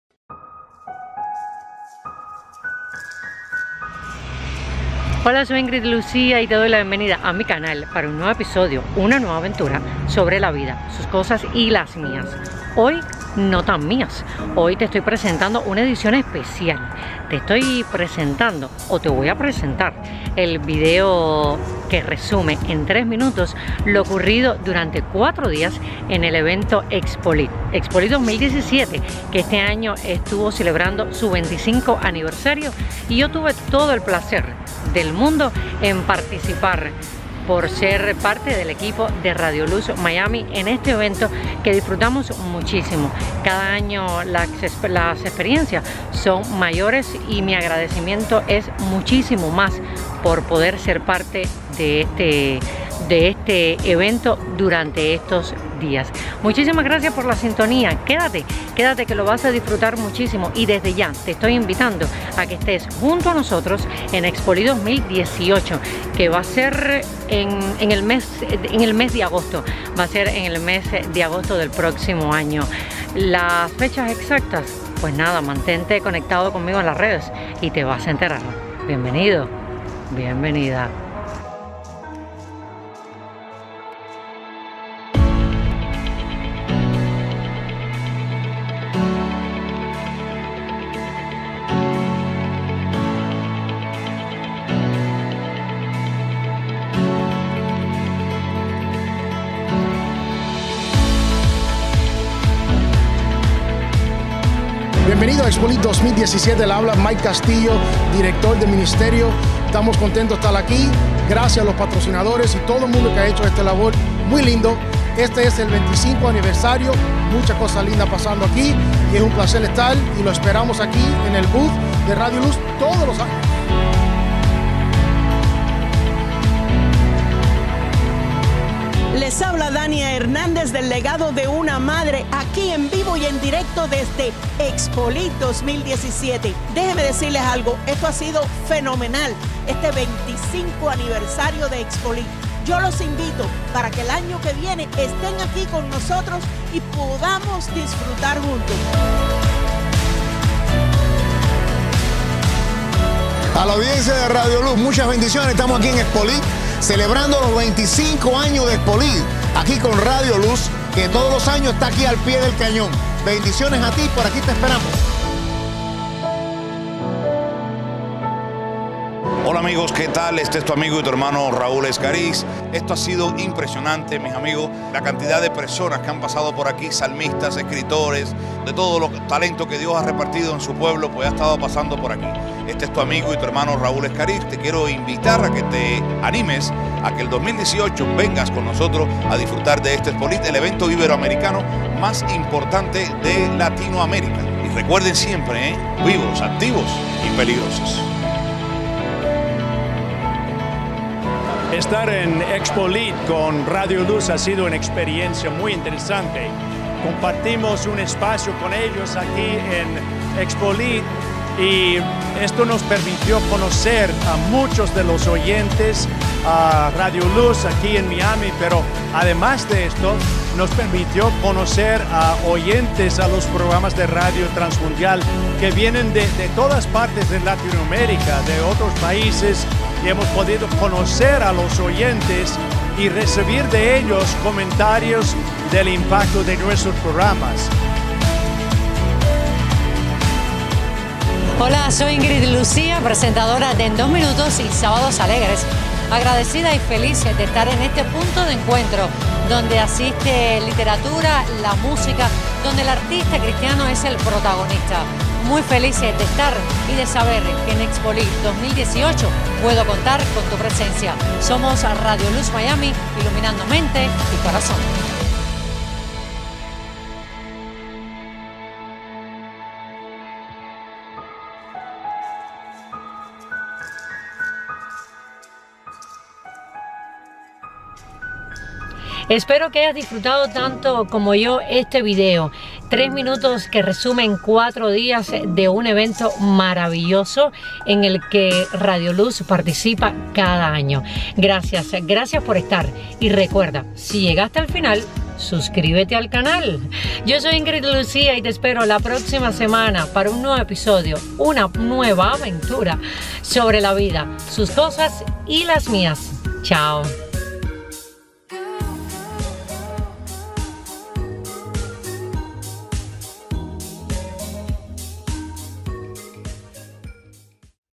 Expolit 2017/Evento
Resumen de 4 dias en Expolit: Convencion Cristiana de habla hispana mas importante que se celebra en Estados Unidos, todo un honor compartir con cada uno de mis compañeros de Radio Luz Miami.